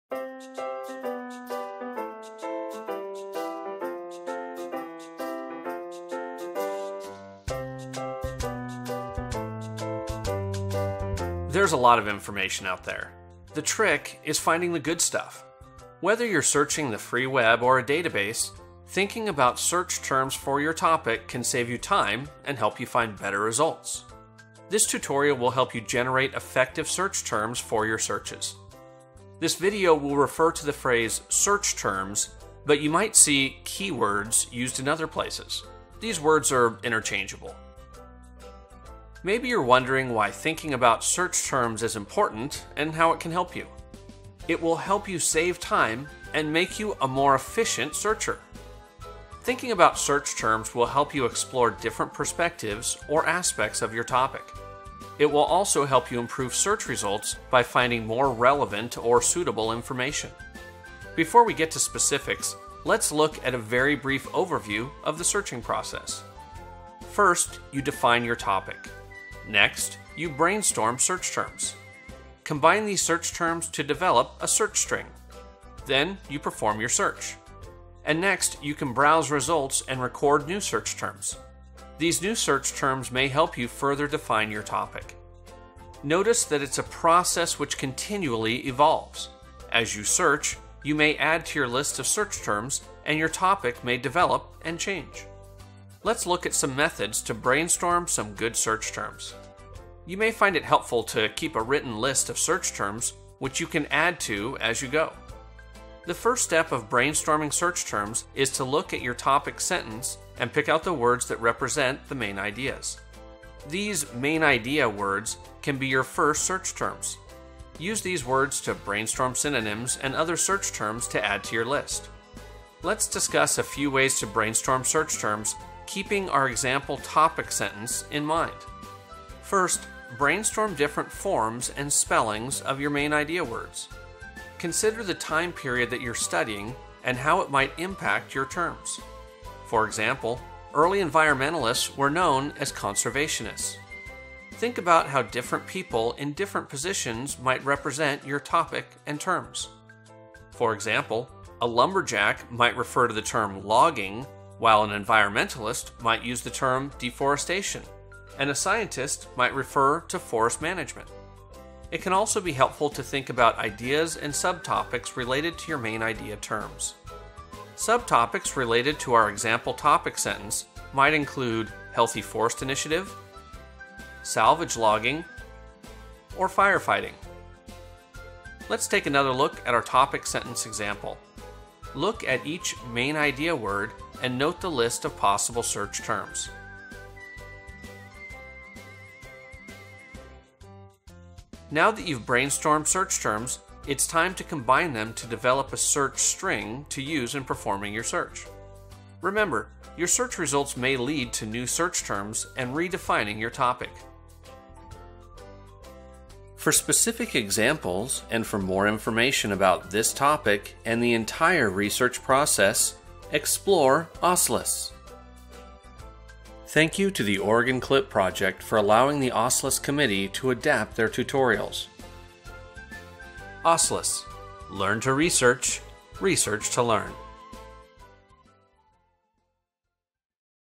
Generating Search Terms
Learn how to determine keywords, develop search strings, and how to find just what you are looking for in a library catalog, database or on the Internet.